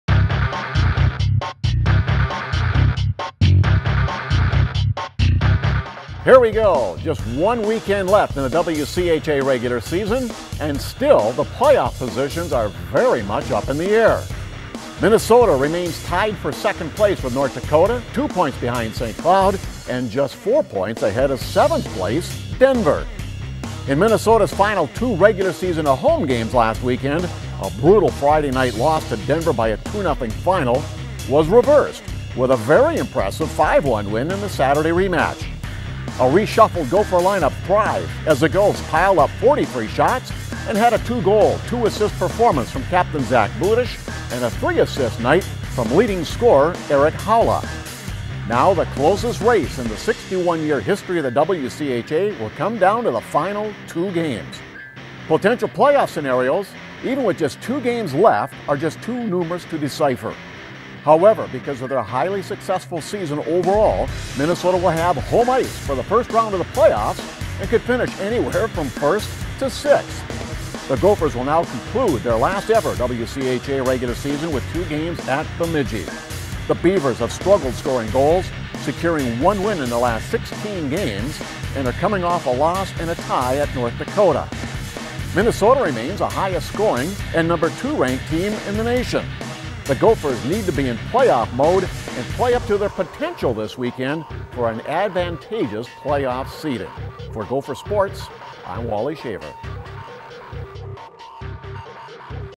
"Wally's Word": Gopher Hockey vs. Bemidji State Preview
Gopher Radio Network voice Wally Shaver breaks down the No. 2 Gopher men's hockey team's March 8-9 series in Bemidji against the Bemidji State Beavers. It's the Gophers final WCHA regular season series before moving to the Big Ten hockey conference in 2013-14.